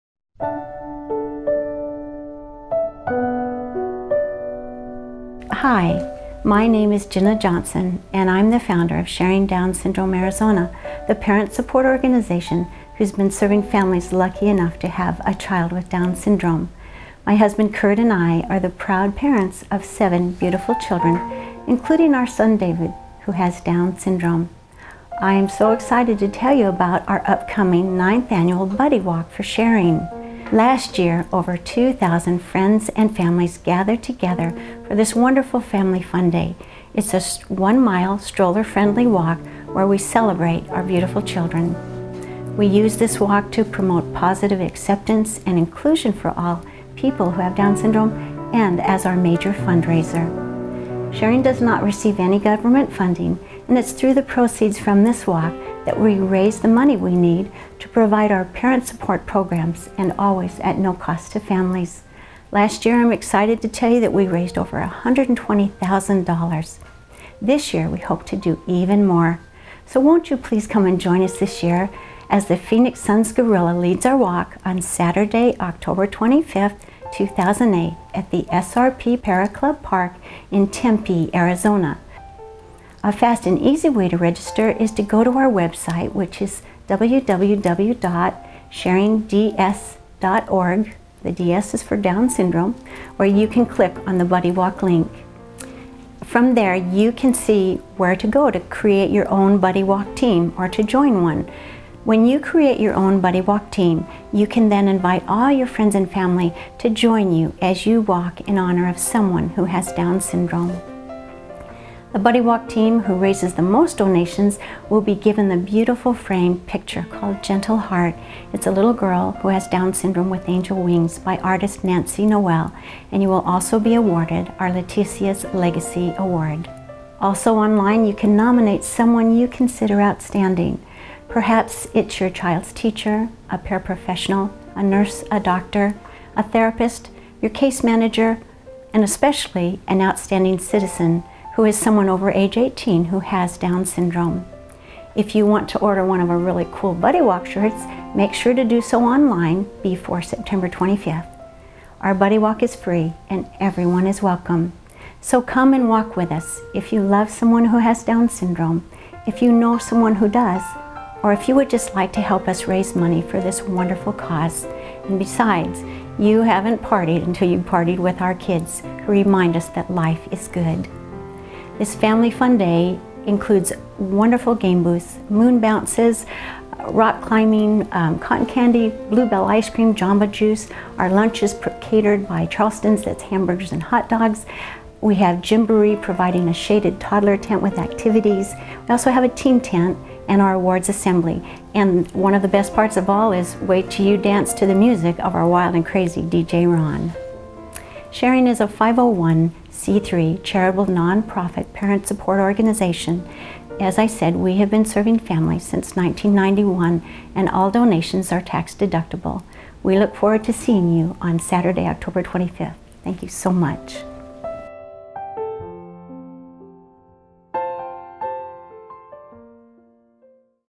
Sharding Down Syndrome of AZ - Buddy Walk
Video about our organization and our Buddy Walk event.